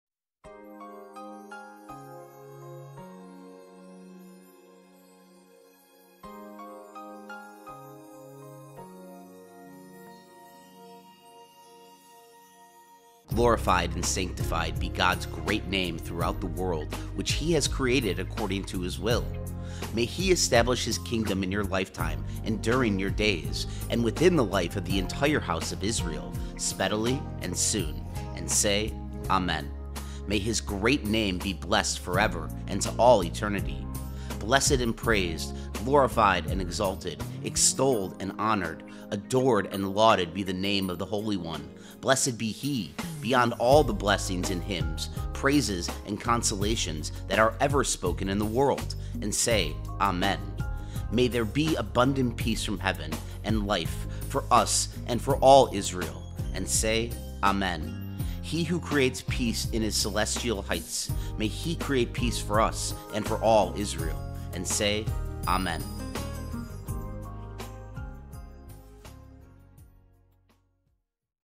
Grieving Prayer(1) Mourner’s Kaddish
A Prayer For those in grief. The Mourner’s Kaddish.